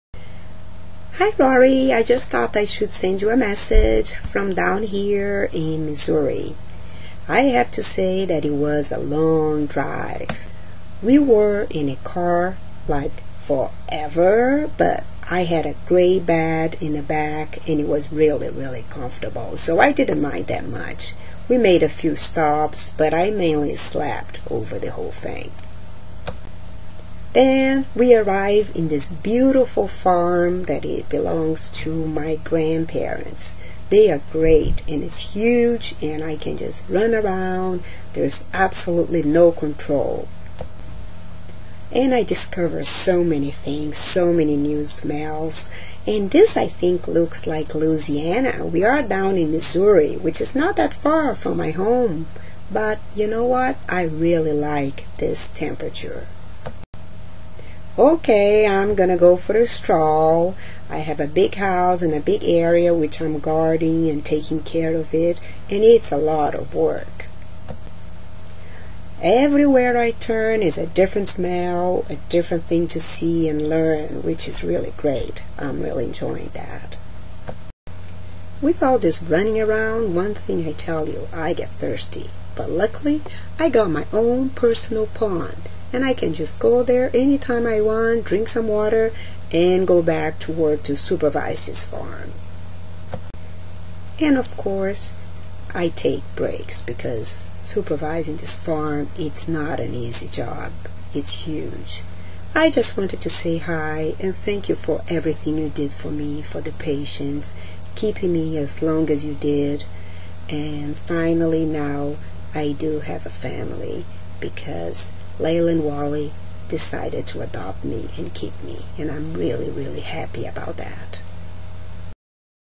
Catahoula Cajun has finnally found a home with the Berg's in Canmore
This is message from Cajun, our adopted Catahoula, from his road trip from Canada to Missouri; dedicated to Rory, who made it possible for him to find a home!